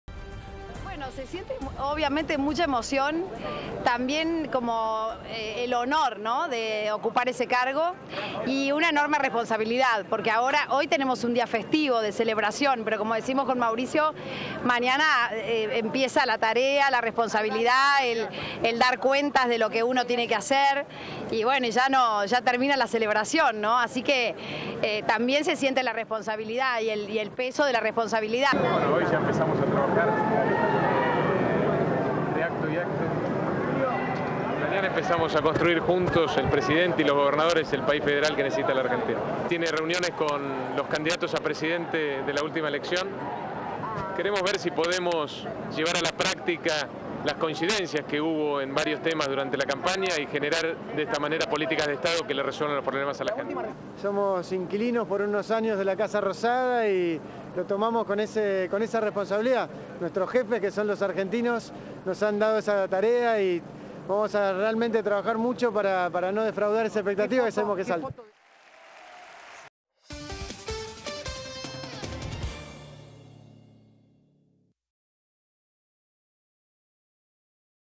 Macri presidente: la gala en el Colón – Telefe Noticias
Más Gabriela Michetti, Rogelio Frigerio y Marcos Peña hablaron con Telefe Noticias en el marco de la gala que tuvo lugar en el Teatro Colón luego de la asunción de Mauricio Macri como Presidente de la Nación. Los presidentes extranjeros en la Argentina El discurso de Macri desde la Casa Rosada